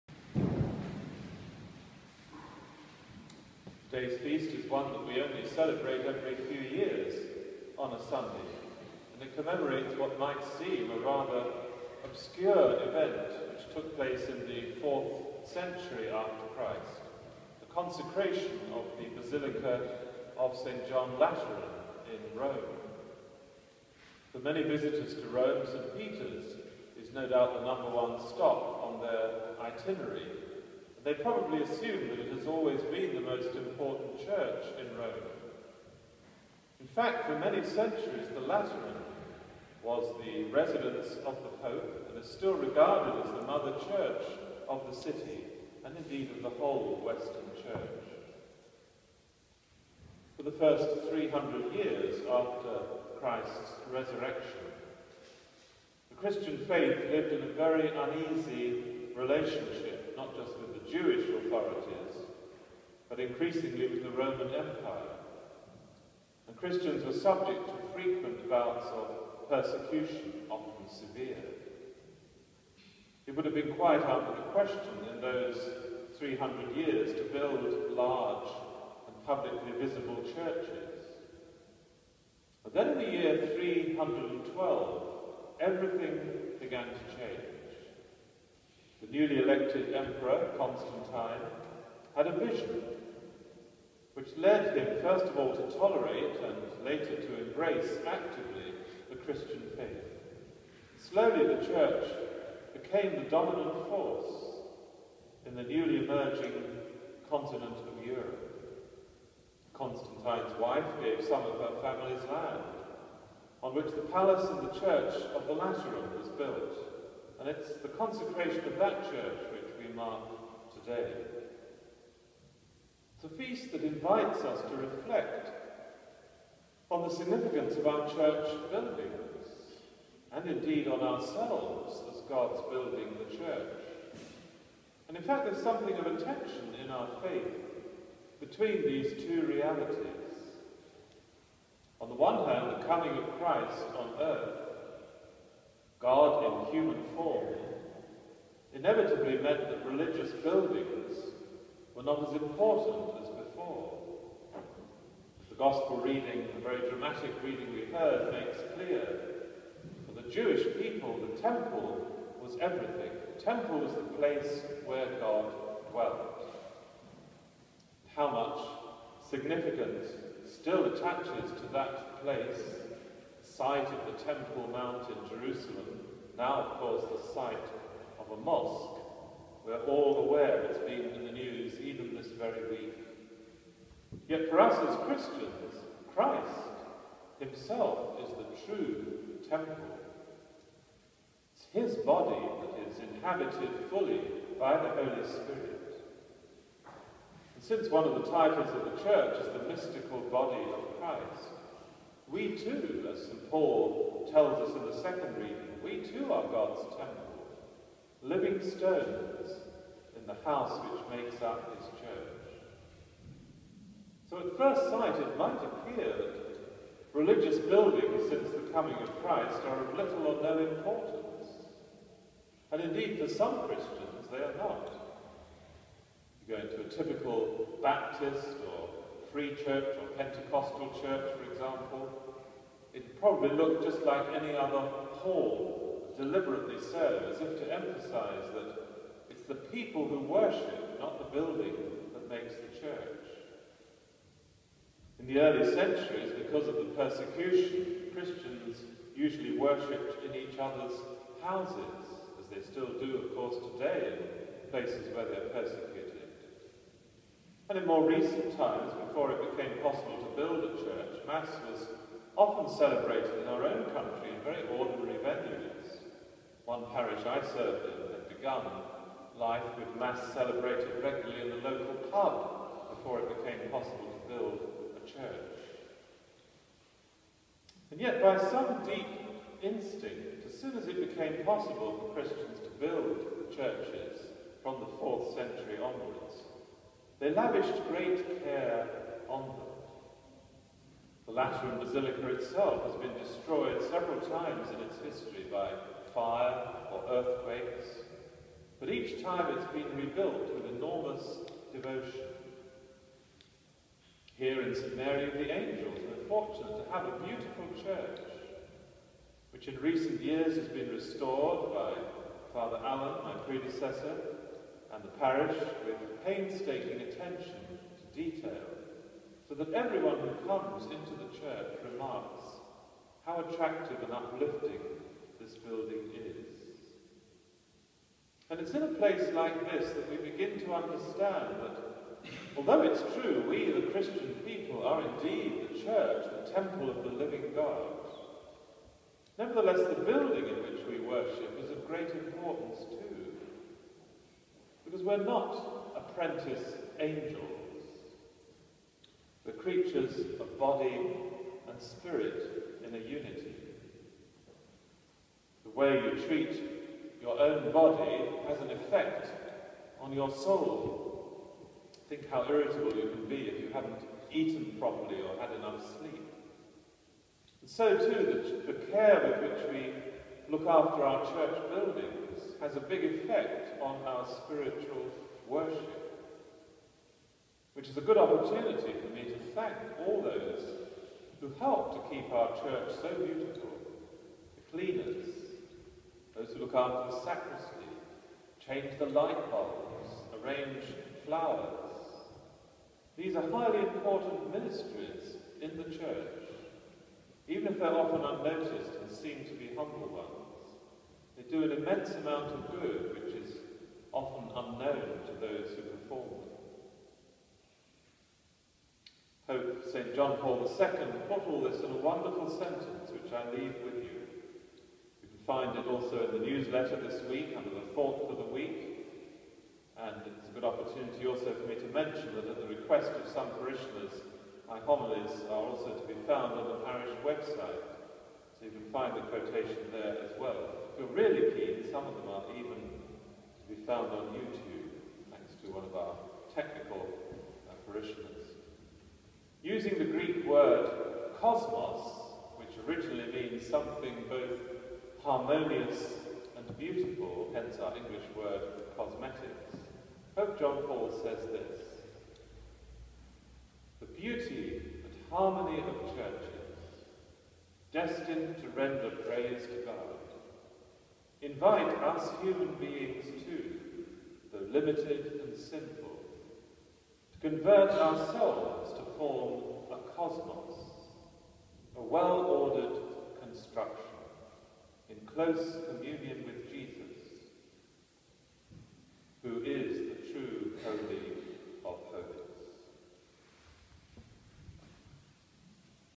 Fr Keith 16 : We are God's Temple
Sunday 9th of November 2014 : Dedication of the Lateran Basilica